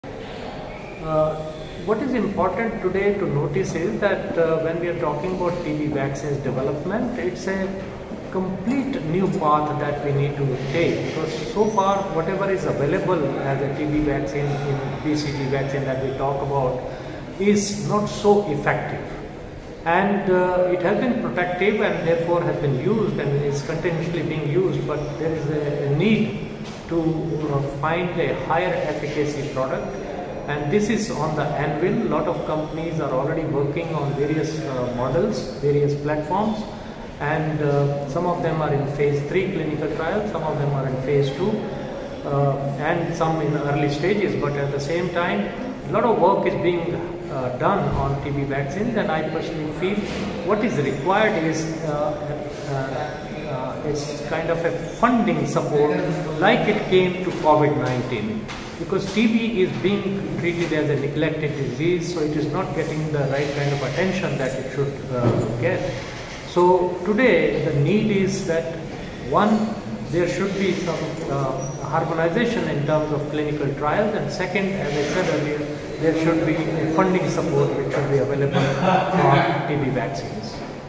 Rajinder Suri, Chief Executive Officer, Developing Countries Vaccine Manufacturers Network (DCVMN)
Rajinder Suri, Chief Executive Officer, Developing Countries Vaccine Manufacturers Network (DCVMN) highlights the need of funding for tuberculosis (TB) vaccine development